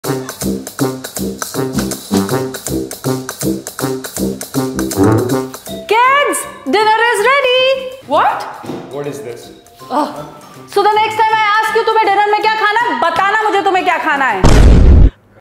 Has your mother ever done this? | Pankaj Bhadouria
Next time be cautious as to what your answer is when Mom asks you:
Aaj Dinner mein kya banaya jaaye?
She might just surprise you!